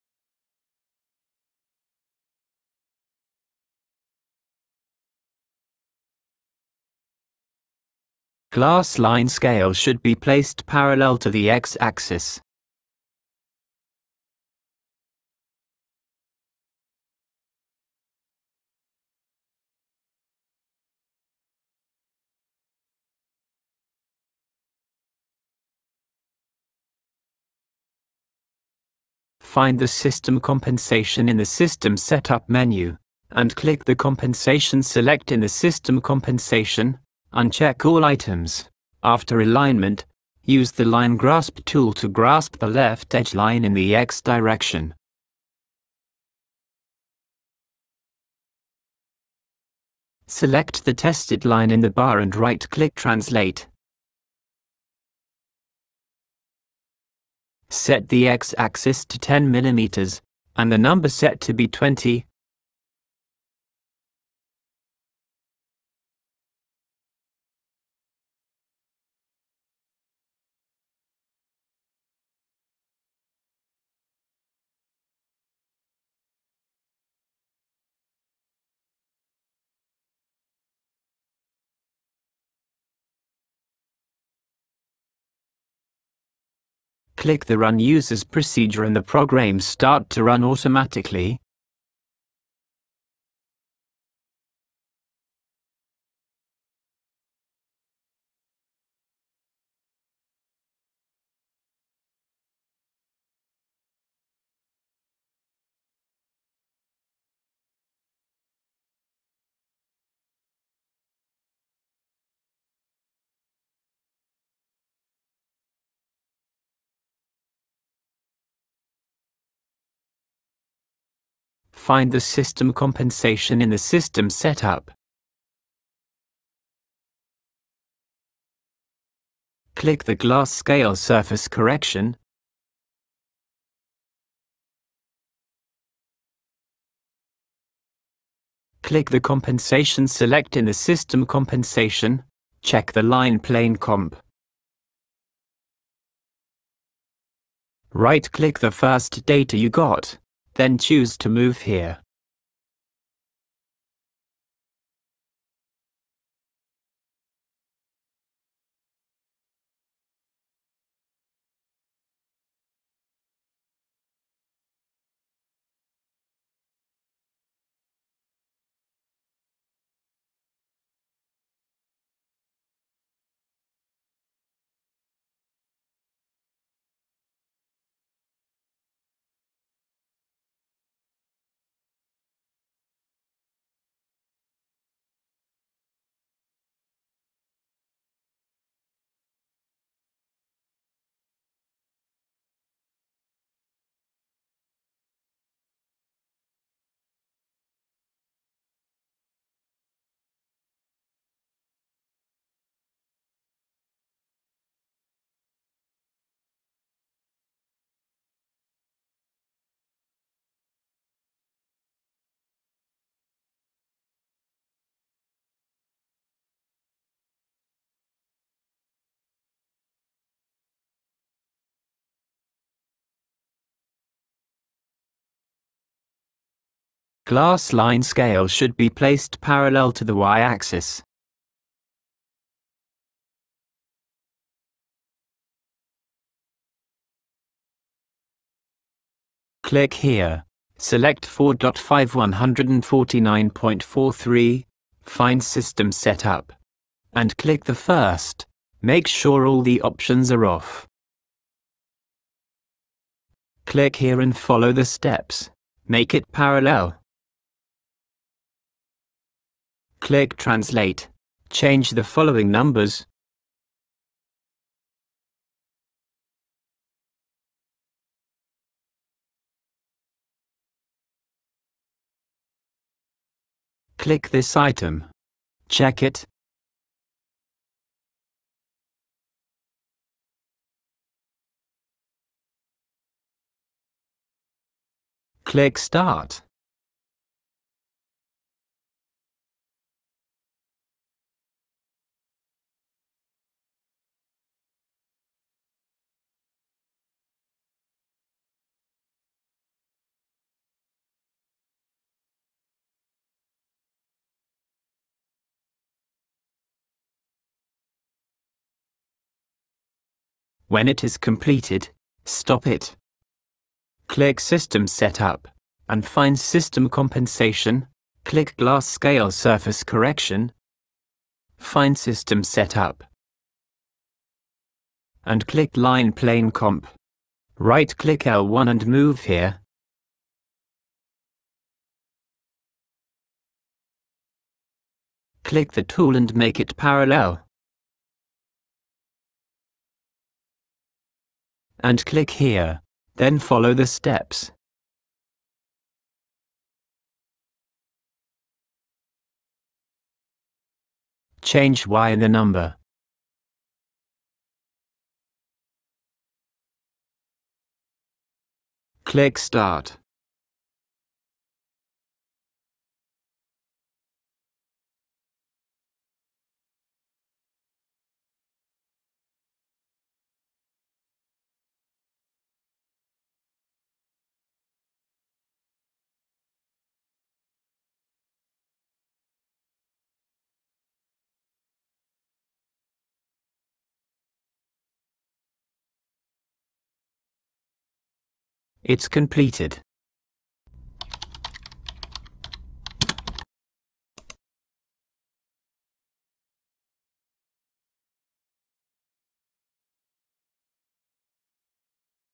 Sinowon-Auto Vision Measuring Machine Calibration
Auto Vision Measuring Machine Calibration
Overview:
Tool preparation and software Setup before calibration
X-axis precision compensation
Y-axis precision compensation
Tips: Ensure the environment and temperature suitable before calibration